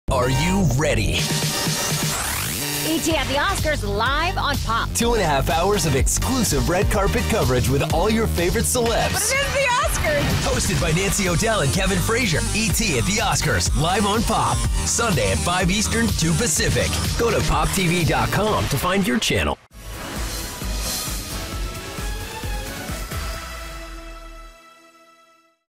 ET and Pop Go on the Red Carpet Together for the 2015 Academy Awards!
With the 87th Academy Awards on Sunday, ET is reteaming with Pop for all of its red carpet coverage!